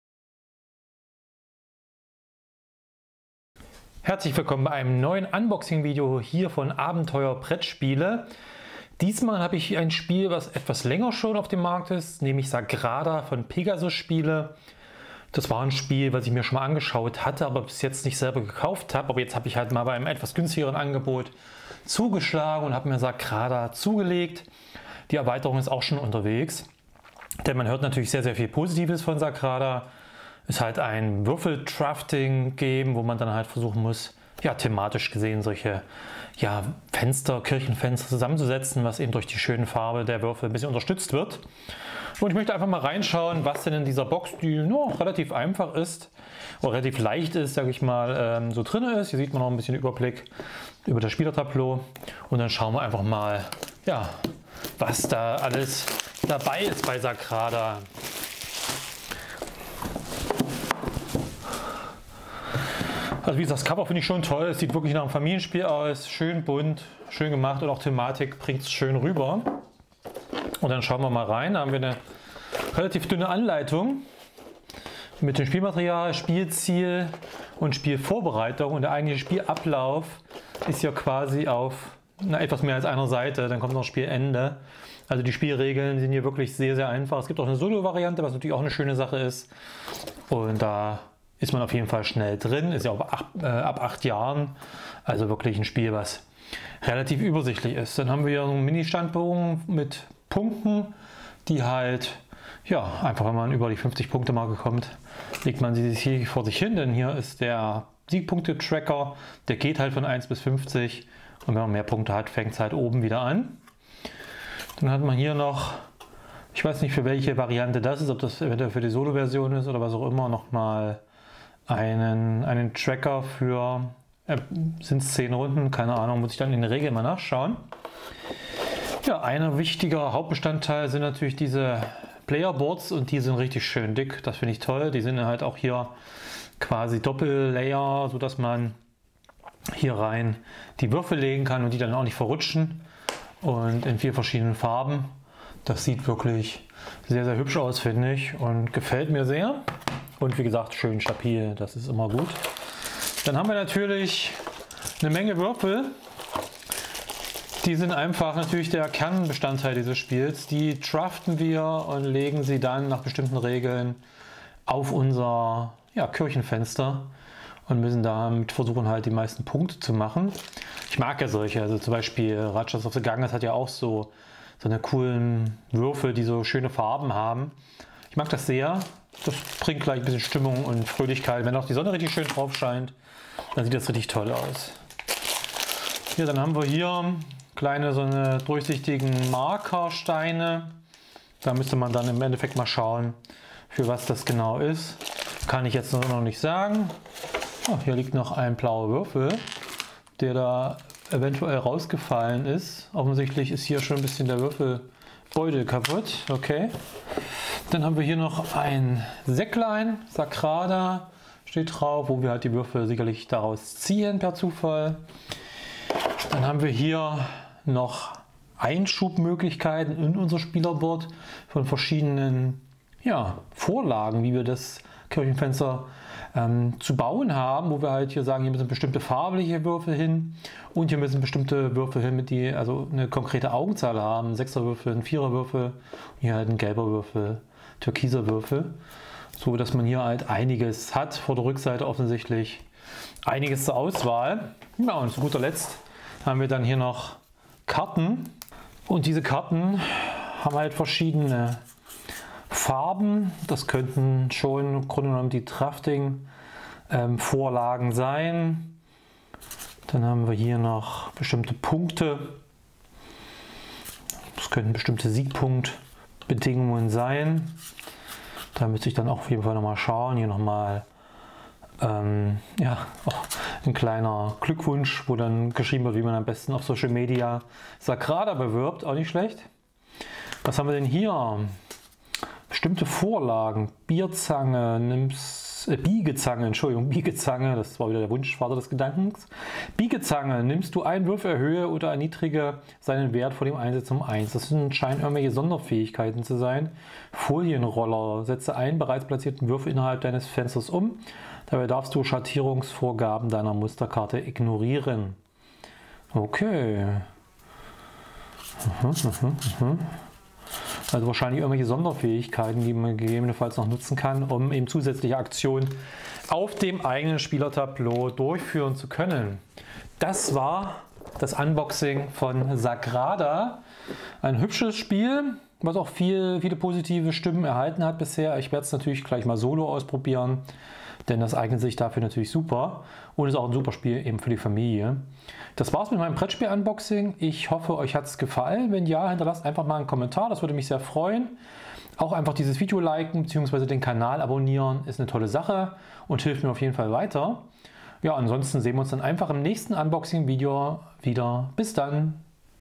Sagrada - Brettspiel Unboxing | Abenteuer Brettspiele
Kirchenfenster basteln wir in diesem Spiel aus bunten Würfeln zusammen und müssen dabei einige Lege-Regeln beachten. Aber wir können auch Sonderaktionen nutzen, um unser "Glasfenster" anzupassen.

Dice Drafting ist hier die Hauptmechanik und in meinem Unboxing-Video schaue ich mir das Spielmaterial genauer an. Viel Spaß damit.